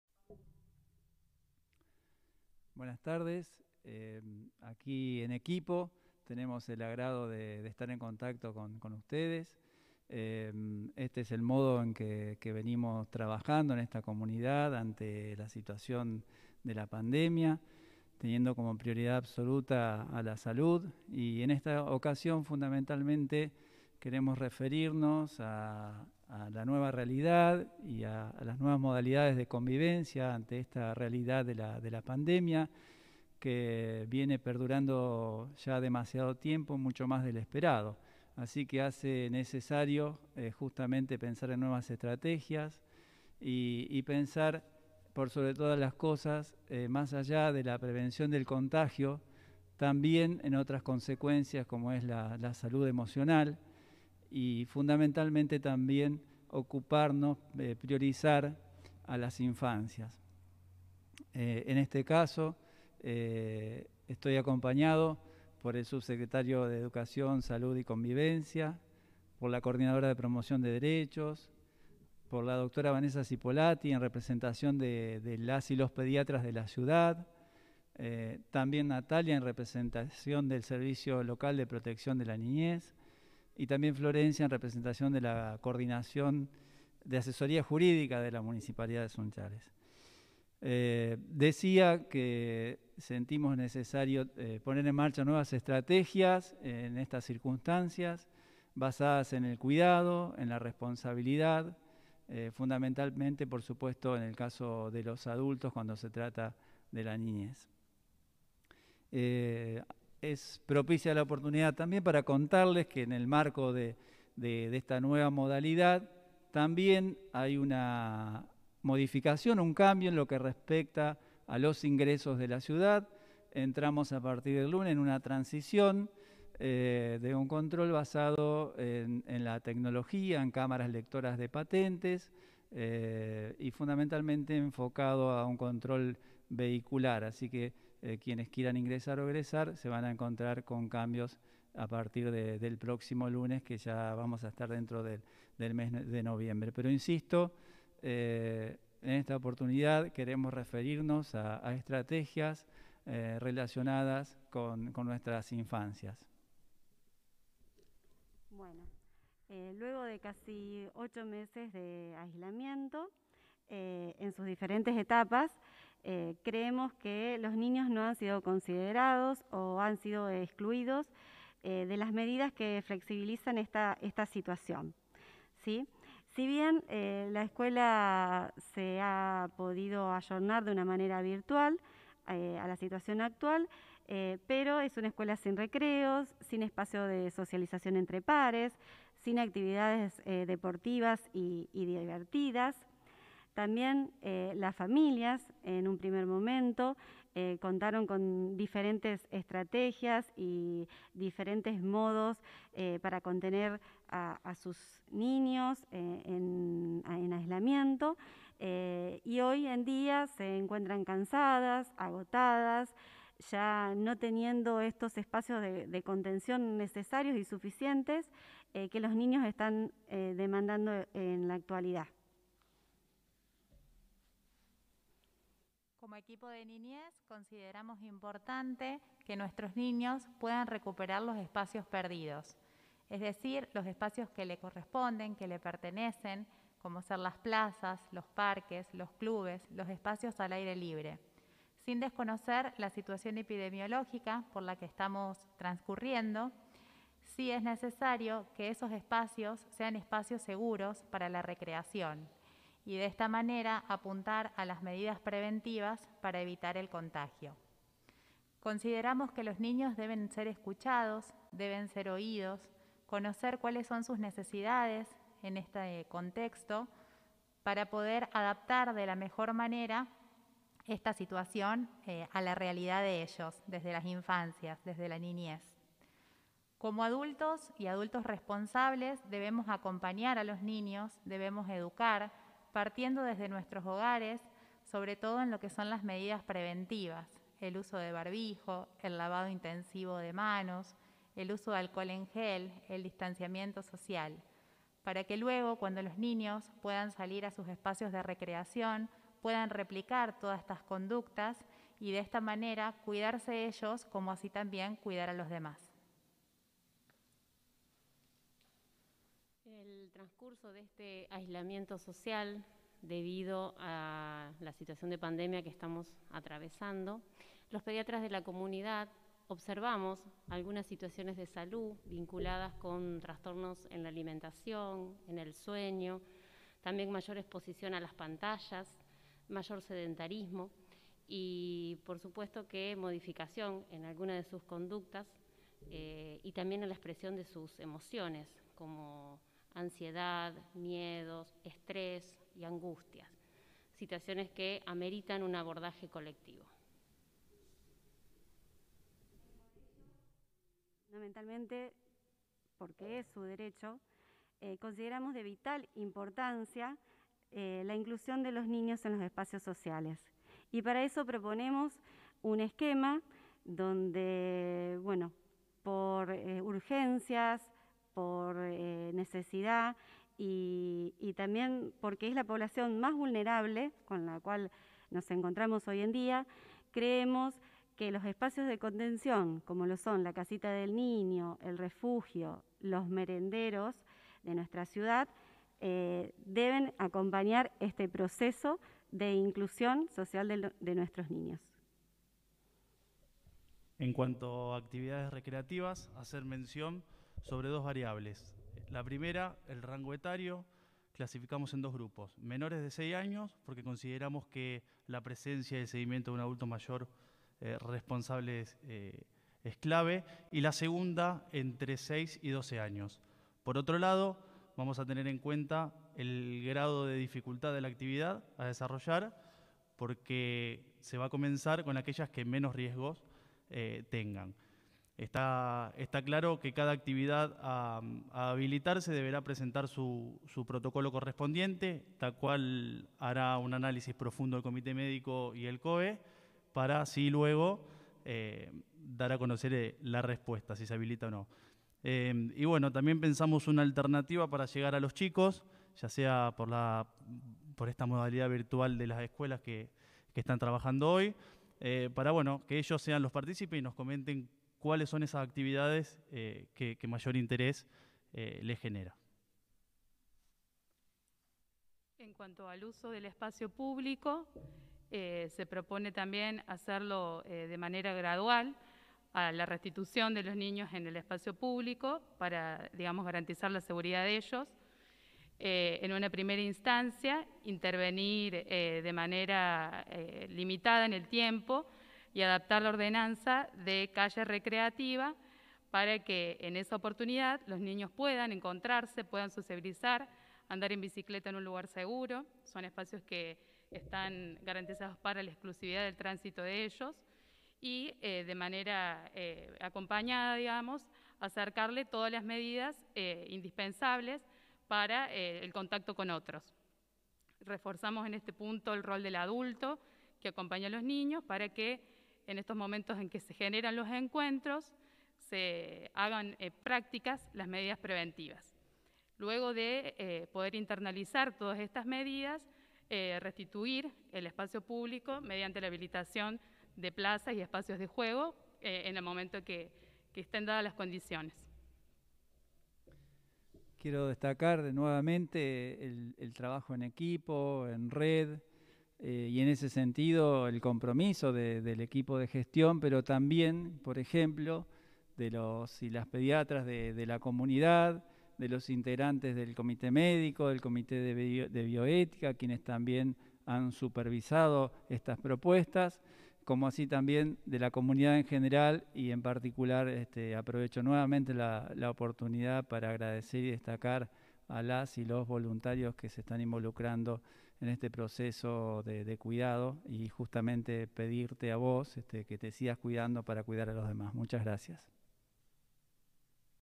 Habilitarán ciertas actividades para niñas y niños bajo estrictos protocolos
Se acordaron disposiciones para pequeños menores a 12 años que comprenden: Activación de  espacios  de  contención priorizando  la  utilización  de  espacios  al  aire  libre; actividades  recreativas fortaleciendo  la  voz  de  las  infancias; y adaptación  y  utilización  de  espacios  públicos.